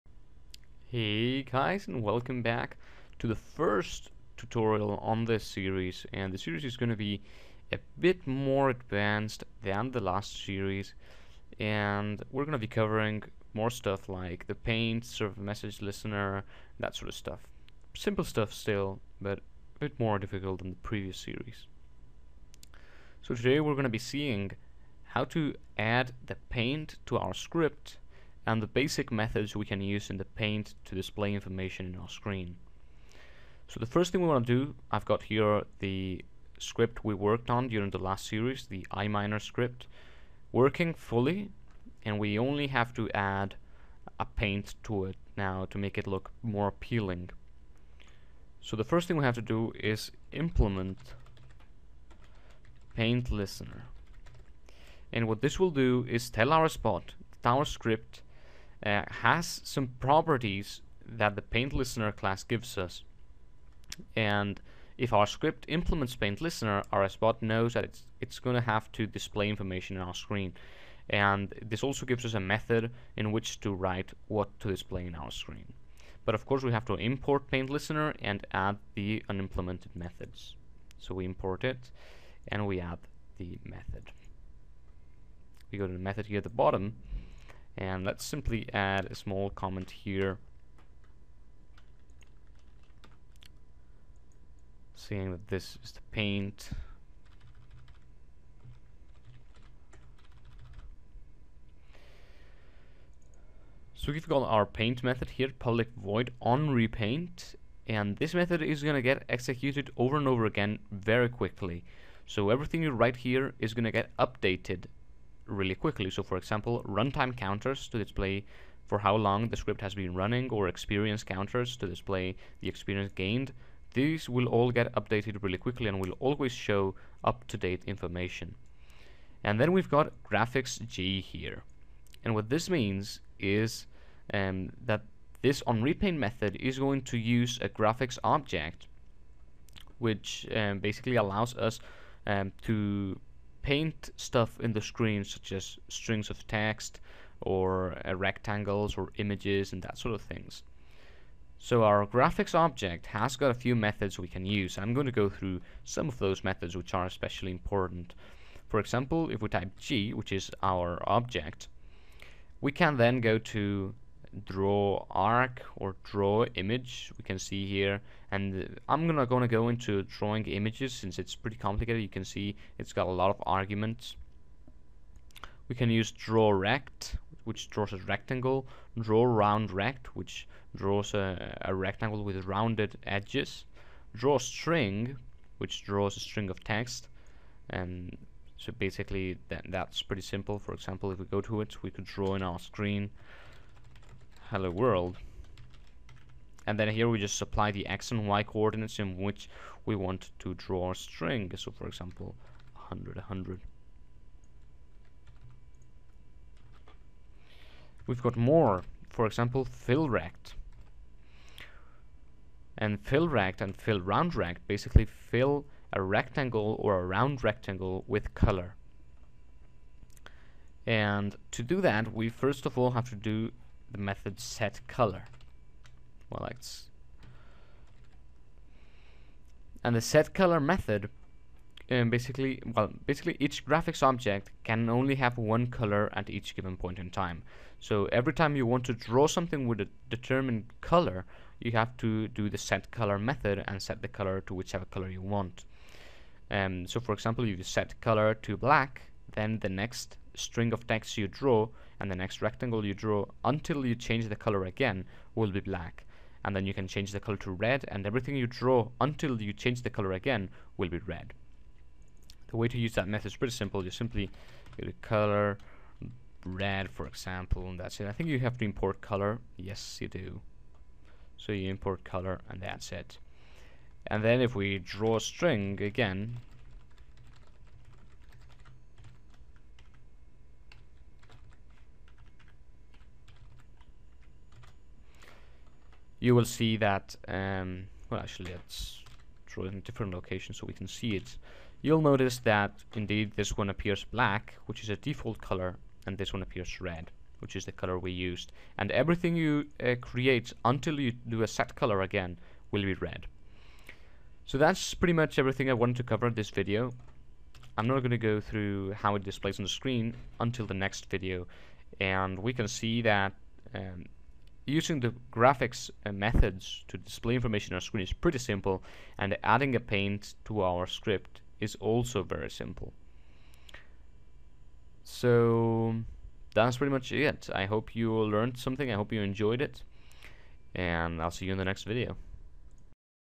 Using Graphics with RSBot
First tutorial in this more advanced series, where we discuss the usage of graphics with RSBot.